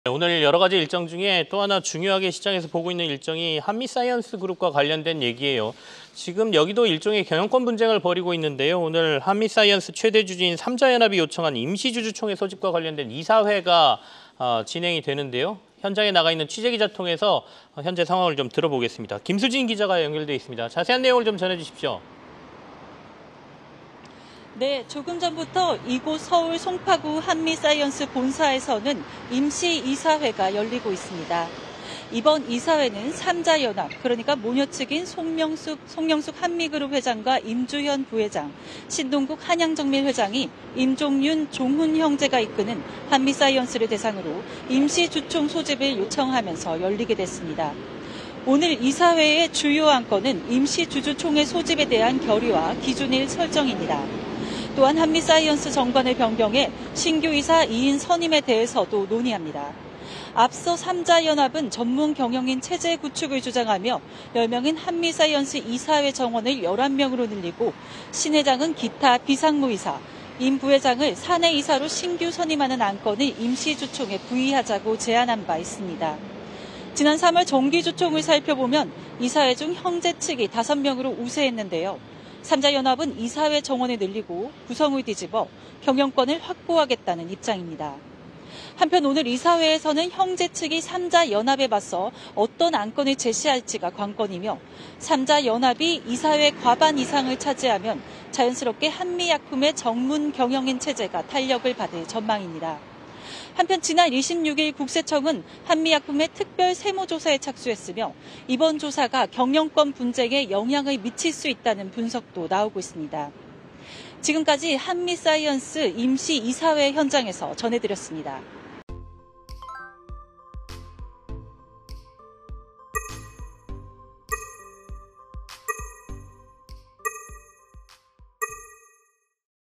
한미, 임주현·신동국 이사 선임·주총 개최 '줄다리기'/한국경제TV뉴스
한미약품그룹의 경영권 분쟁이 10개월 가까이 이어지는 가운데, 오늘 한미사이언스가 임시 이사회를 개최합니다. 한미사이언스 최대 주주인 '3자 연합'이 요청한 임시주주총회 소집을 위한 이사회인데요. 현장에 나가 있는 산업부 김수진 기자 연결해 자세한 소식 살펴봅니다.